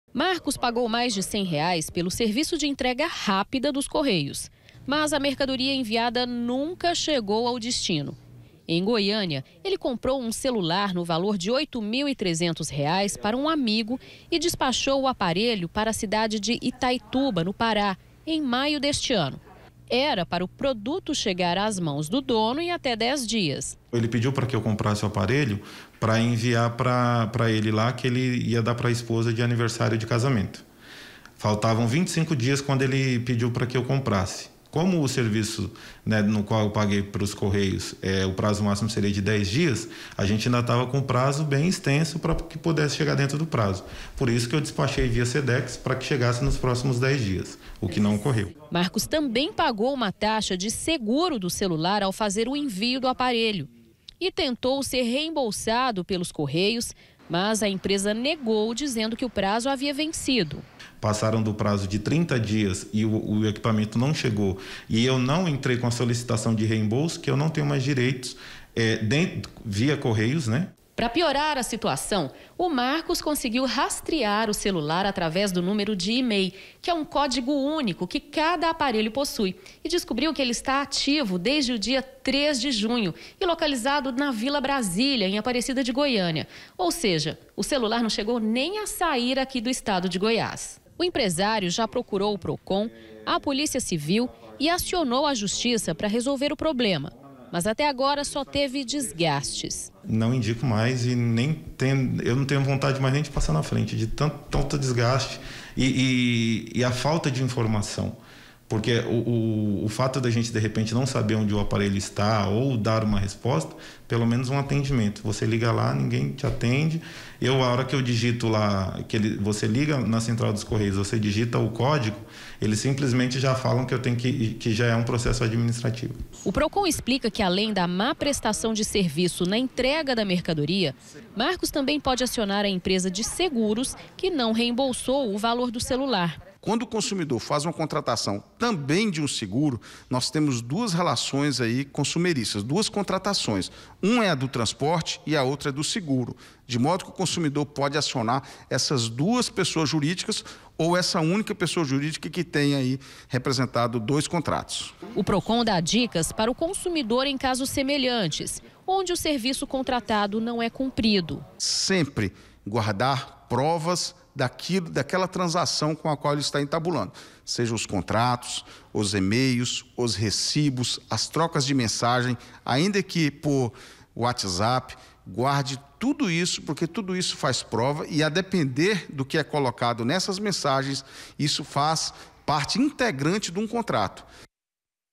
CELULAR DE R$ 8 MIL É EXTRAVIADO DURANTE ENTREGA
UM EMPRESÁRIO CONTRATOU O SERVIÇO DOS CORREIOS PARA ENTREGAR UM CELULAR DE  MAIS DE OITO MIL REAIS, EM DEZ DIAS E TEVE ENTREGA EXTRAVIADA. ISSO FOI EM MAIO, E ATÉ HOJE ELE NÃO TEVE INFORMAÇÃO DO QUE ACONTECEU.